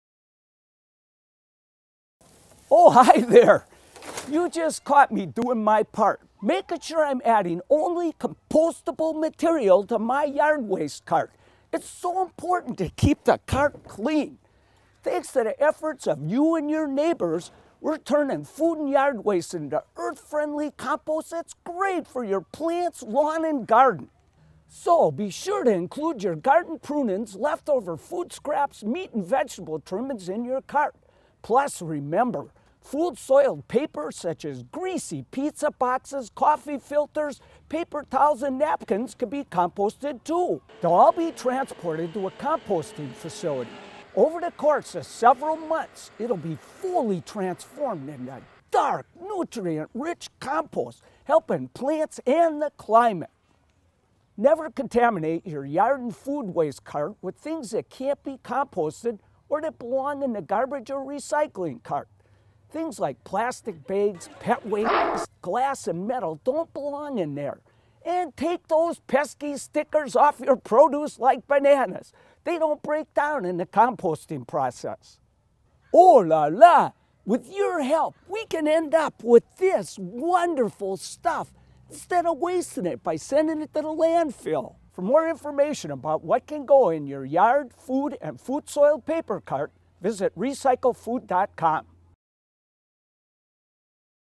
Composting Tips from Ciscoe Morris
Find out what can and cannot go in your curbside Yard Waste Cart from gardening expert Ciscoe Morris!